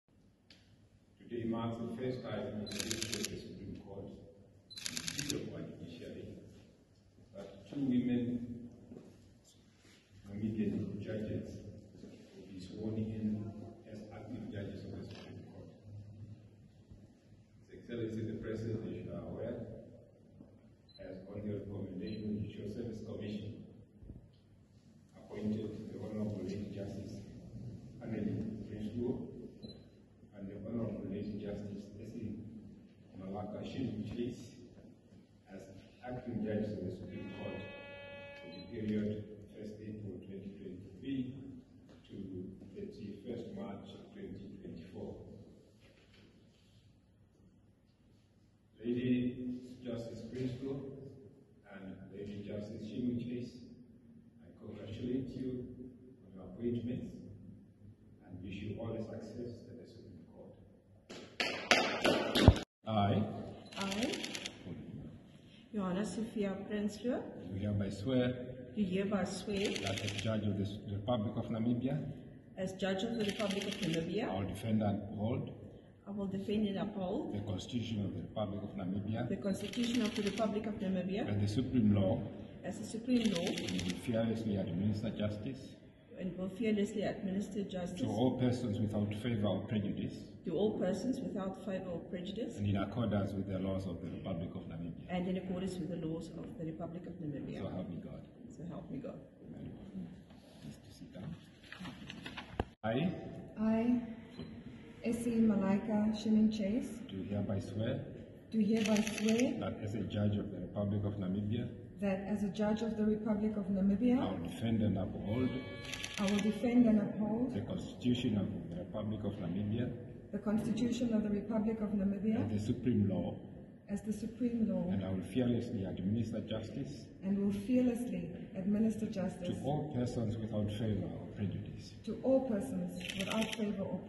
Landmark Moment: Namibia Appoints First Female Acting Judges to Supreme Court
The first Namibian women appointed as acting judges of the Supreme Court, judges Hannelie Prinsloo and Esi Schimming-Chase, were sworn in at the Supreme Court today. Chief justice Peter Shivute administered the oaths of office to the two judges.
Videos: Werner Menges